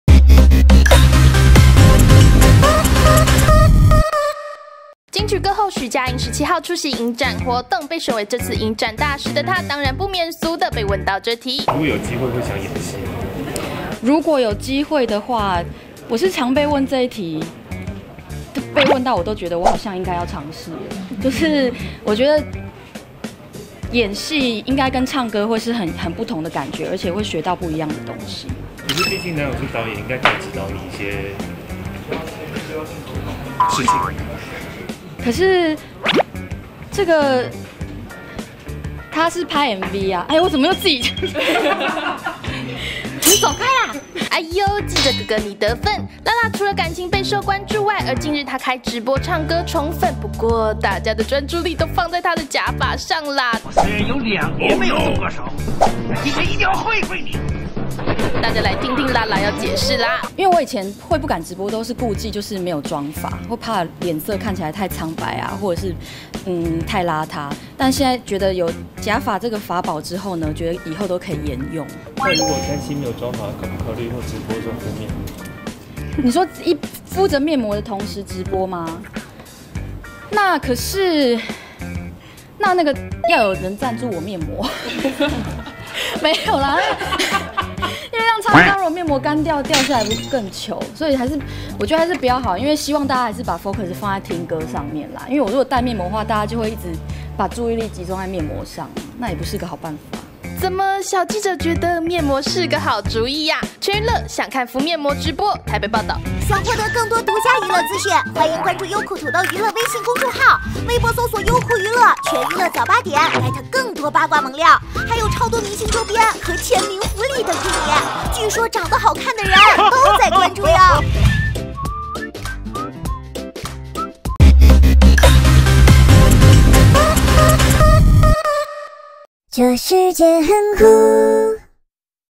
【2年前】港臺：徐佳瑩直播戴法寶 假髮擋住沒妝發窘境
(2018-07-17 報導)

金曲歌后徐佳瑩
17號出席影展活動
被選為這次影展大使的她
當然不免俗的被問到這題